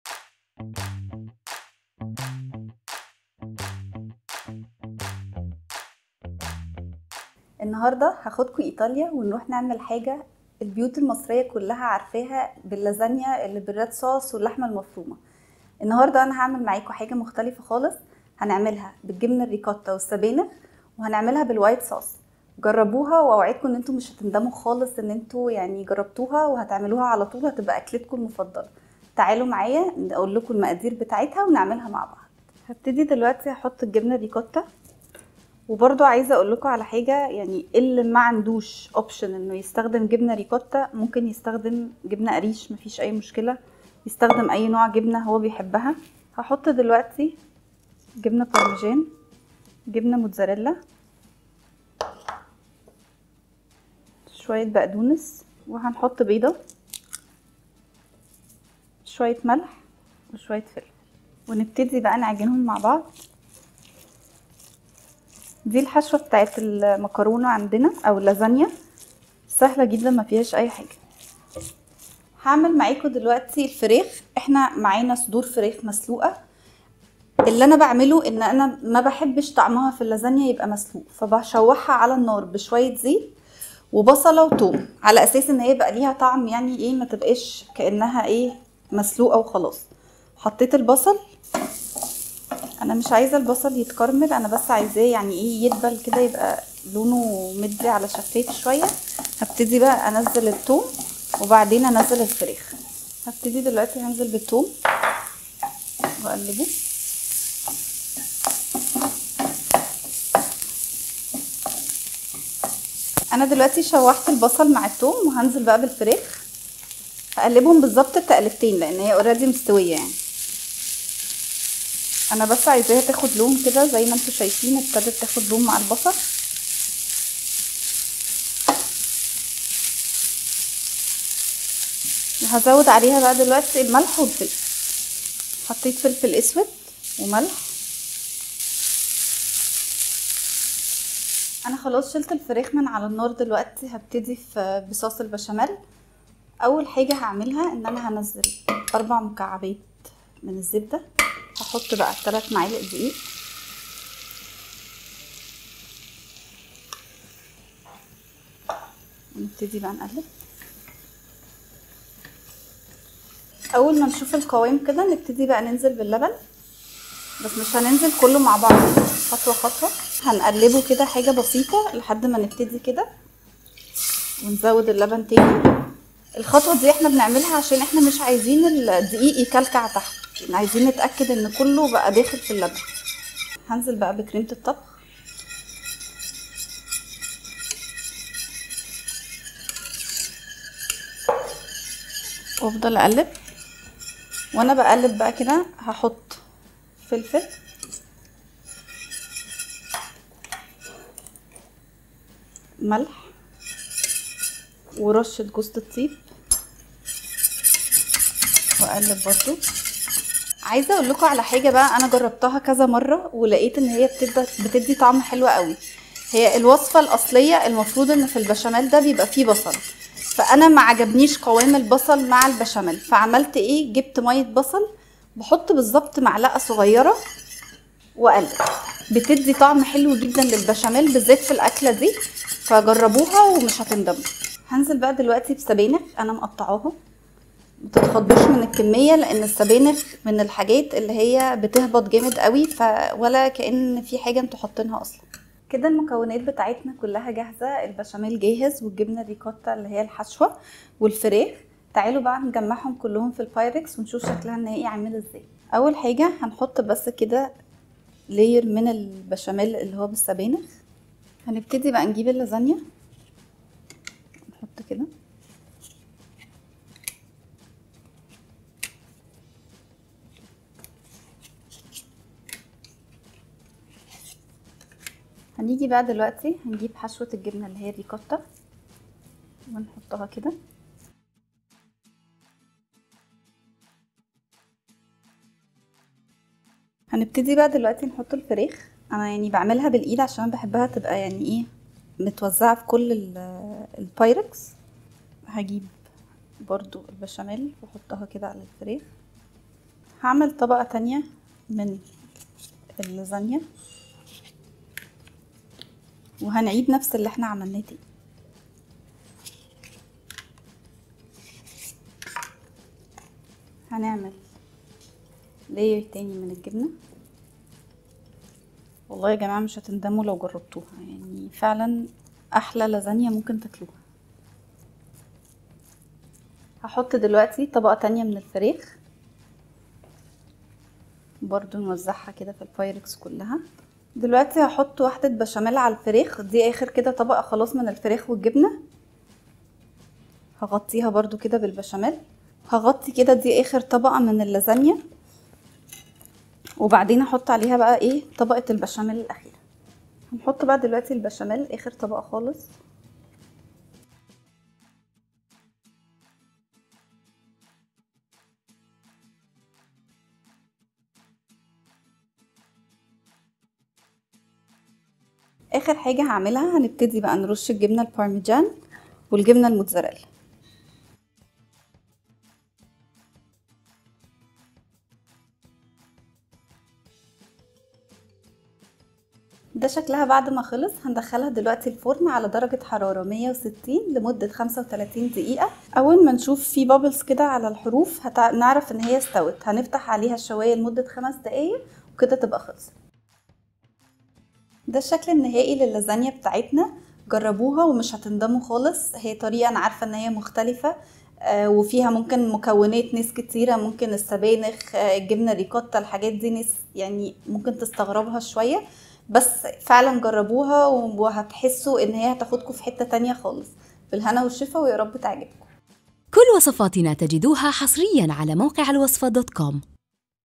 لازانيا بالطريقه الايطالي بالوايت صوص وجبنه الريكوتا والفراخ
كل التفاصيل من هنا
المقادير
1 كيلو دجاج مسلوق-مخلي
1 باكيت لازانيا
3 حبة بصل مقطع صغير
3 فص ثوم مهروس
1 كيلو حليب
1 كوب كريمه طبخ
1 كوب سبانخ
1 علبة جبن ريكوتا
1 كوب جبن موتزاريلا
كوب جبن بارميزان
1 عدد بيض
ملح حسب الرغبة
فلفل ابيض حسب الرغبة
¼ معلقة صغيرة جوز الطيب
1 كوب بقدونس مفروم للتزين
4 مكعب زبدة
3 معلقة كبيرة دقيق